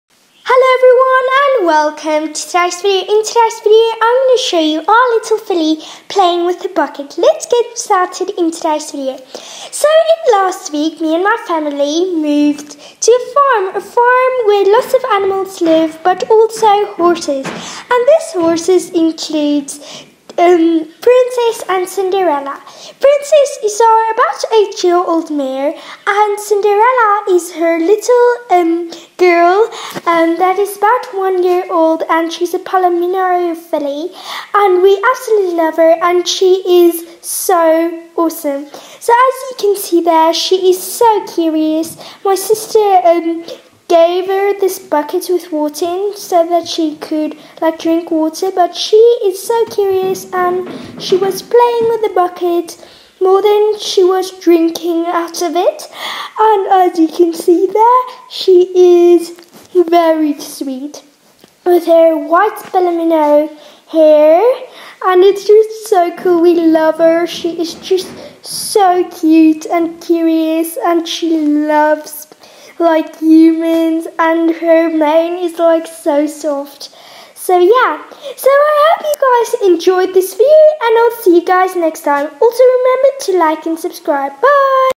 Our little filly playing with a bucket!
Our little Palomino filly playing joyfully with a bucket!